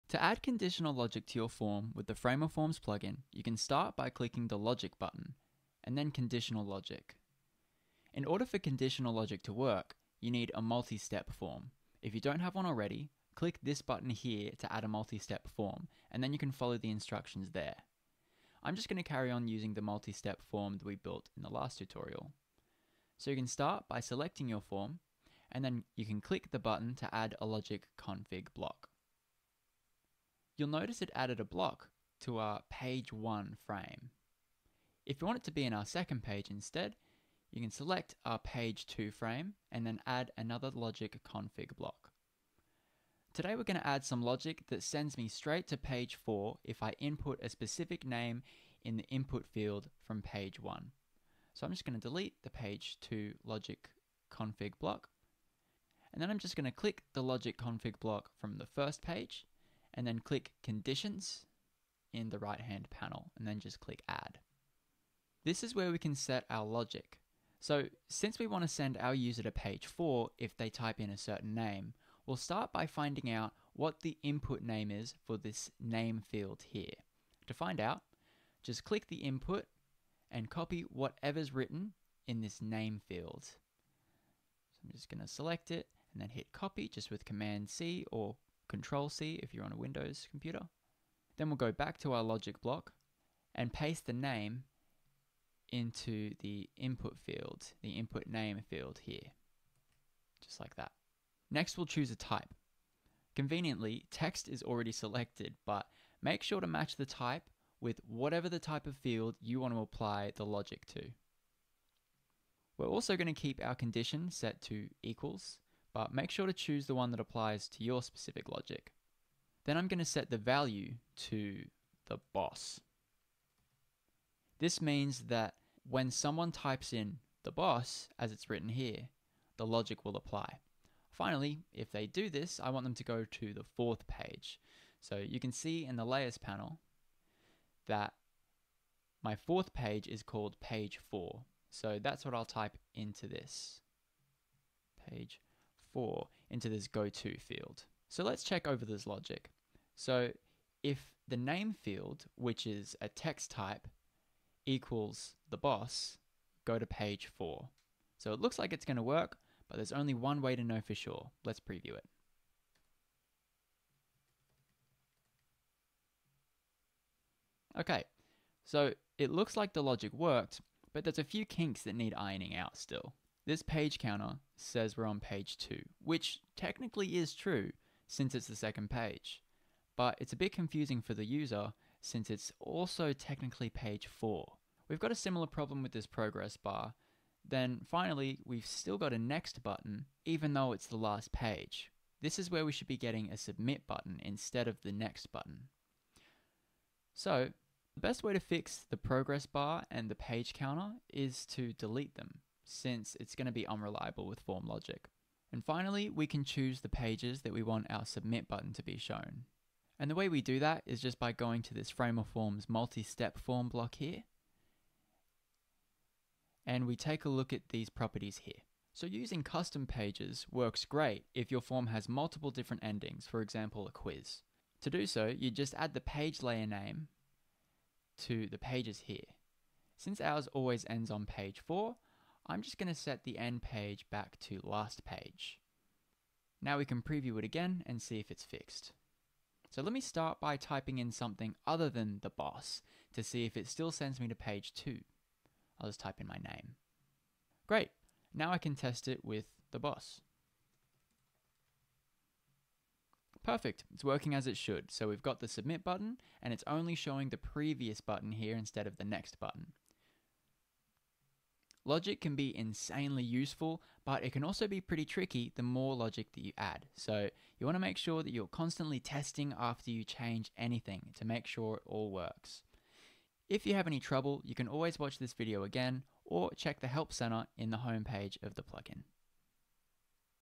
How to Add Conditional Logic to a Framer Form (FramerForms Plugin)
In this video, Wes shares how to add conditional logic to a framer form using the FramerForms plugin.

Want more Framer plugins and products? 👉 insertframe.io